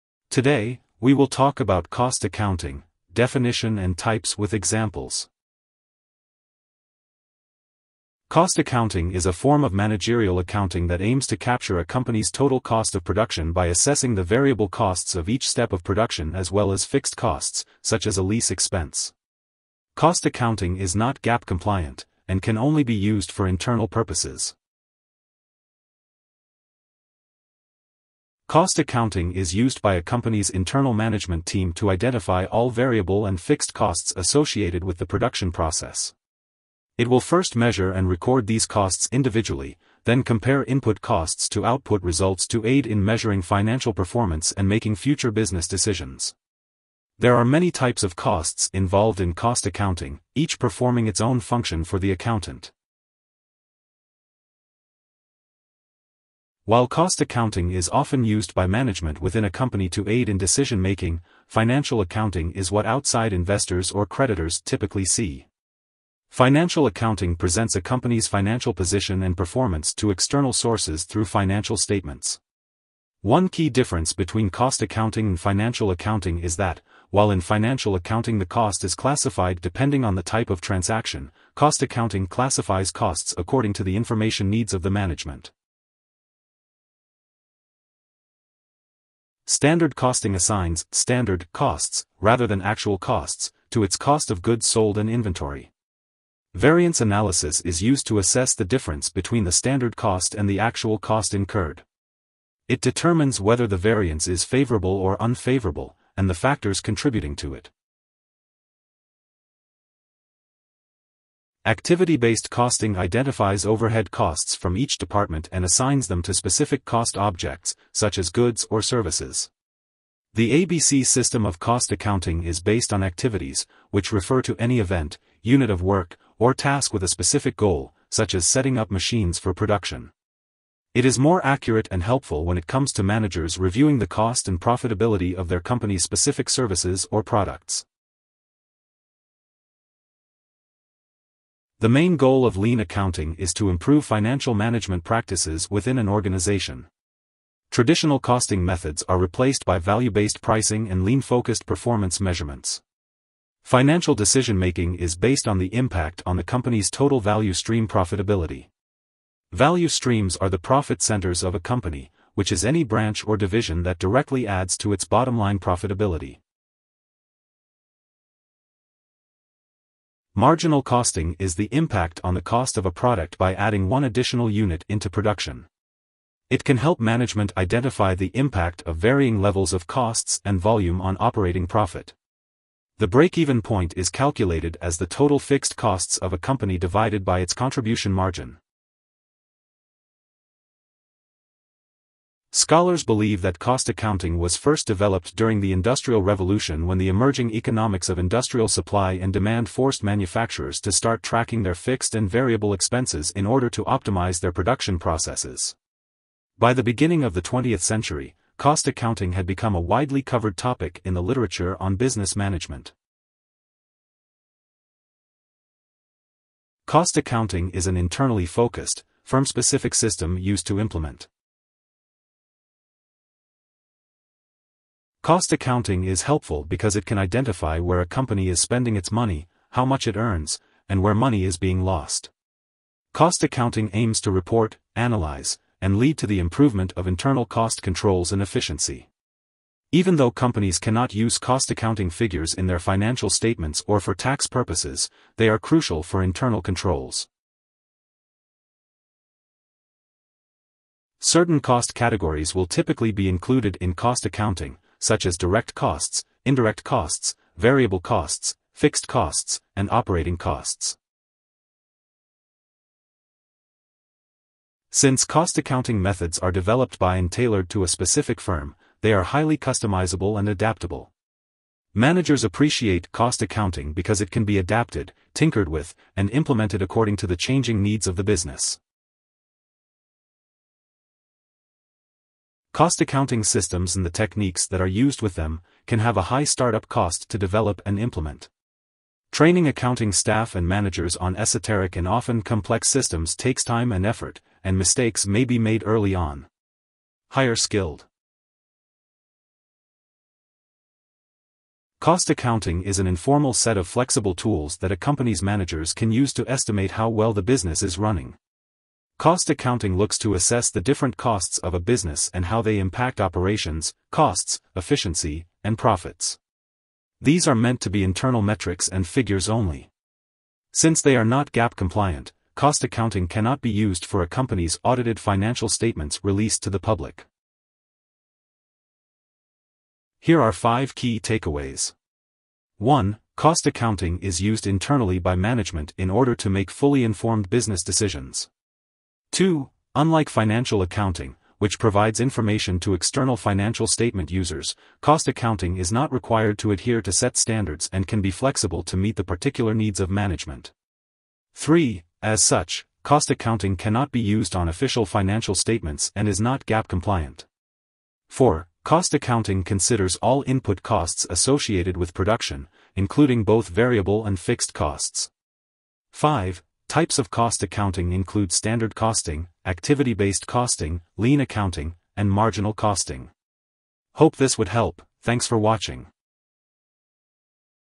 Cost Accounting: Definition and Types With Examples
Cost accounting is an informal set of flexible tools that a company's managers can use to estimate how well the business is running. This form of managerial accounting captures a company's total cost of production by assessing variable and fixed costs. Cost accounting, not being GAAP-compliant, is used for internal purposes. It helps companies identify where money is being spent and how much it earns, leading to the improvement of internal cost controls and efficiency.
8525